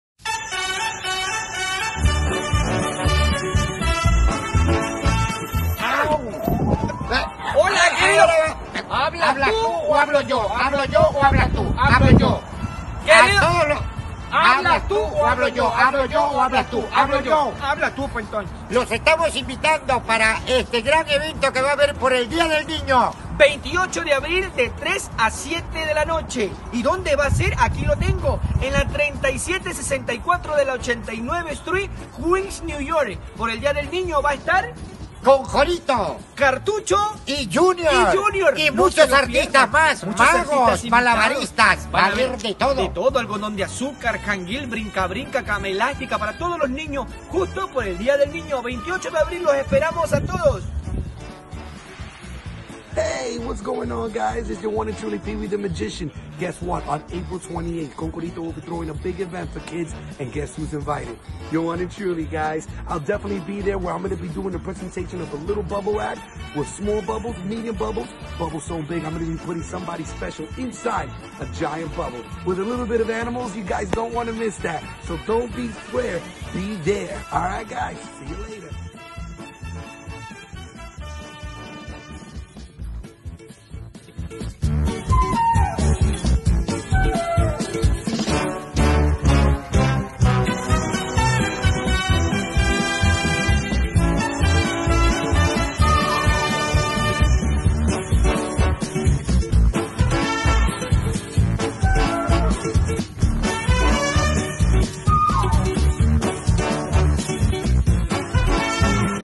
Celebrando El Dia Del Niño
Circo Navas Celebrando el Dia del Niño el 28 de Abirl
En Queens,NY con Cartucho & JR.